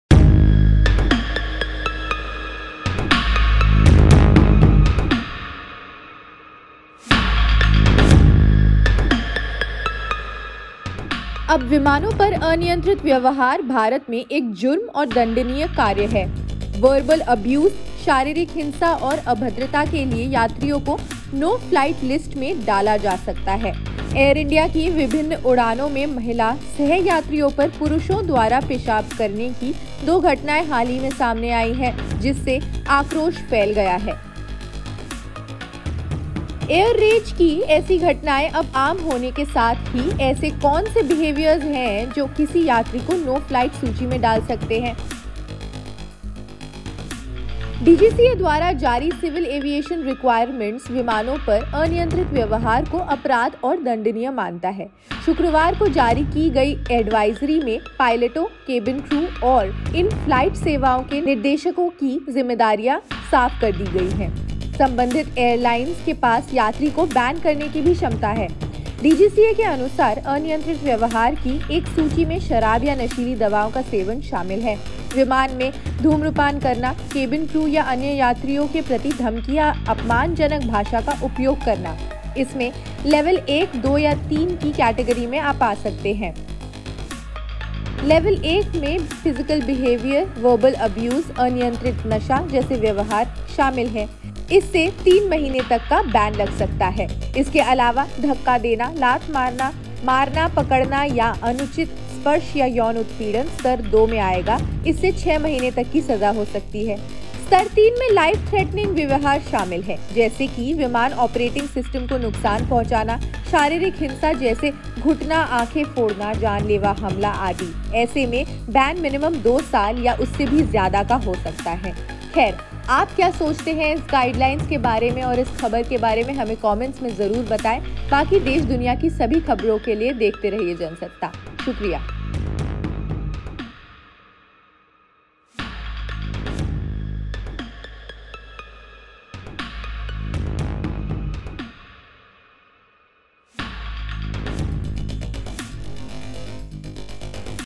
Urinating Men Air India: DGCA ने अनियंत्रित यात्रियों से निपटने के लिए एडवाइजरी जारी की!| Guidelines
DGCA Advisory Air India: अब विमानों पर अनियंत्रित व्यवहार भारत में एक जुर्म और दंडनीय कार्य है। वर्बल एब्यूज (Verbal Abuse), शारीरिक हिंसा (Physical Violence) और अभद्रता (Indecency) के लिए यात्रियों को नो फ्लाई लिस्ट (No Flight List) में डाला जा सकता है। एयर इंडिया (Air India Flights) की विभिन्न उड़ानों में महिला सह-यात्रियों पर पुरुषों द्वारा पेशाब करने की दो घटनाएं हाल ही में सामने आई हैं, जिससे आक्रोश फैल गया है. एयर रेज की ऐसी घटनाएं अब आम होने के साथ ही, ऐसे कौन से व्यवहार हैं जो किसी यात्री को नो-फ्लाई सूची में डाल सकते हैं.